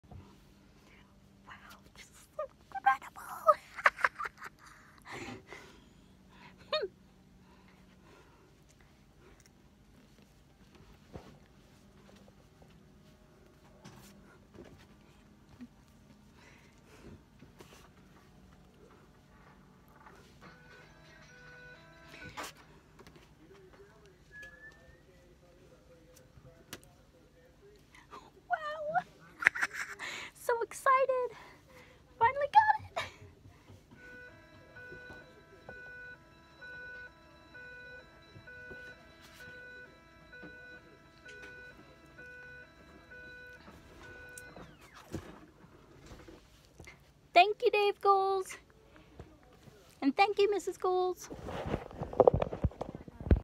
Thank You, Dave Goelz!!!!!! - My Autism Journey
Today I received a wonderful package in the mail from Dave Goelz. His wife Debra had messaged me via Twitter a week ago, and she told me that Dave had read my birthday message that I sent him and loved it. I was so excited, and I checked the mail every day last week to see if it came. Today was the day that it came!!! I was so excited that I got it!!! Thank you Dave Goelz for the autographed pictures, and thank you Debra Goelz for showing the birthday message for Dave!!!